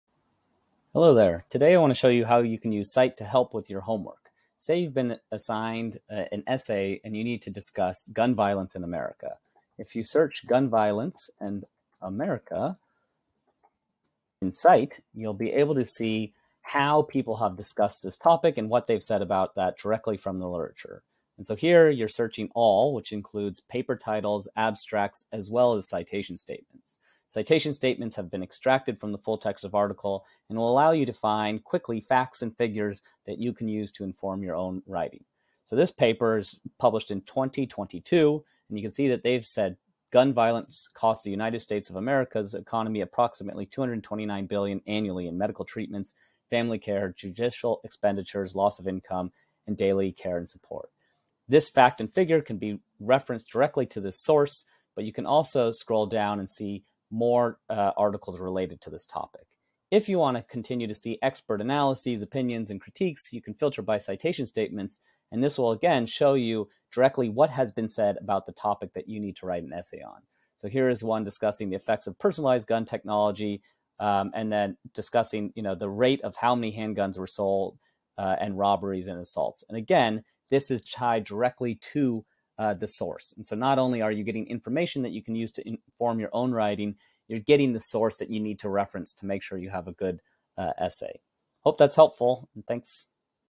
How Scite can help you with your homework
A short video documenting how scite can help you better understand any research topic, find relevant sources, and write your papers better.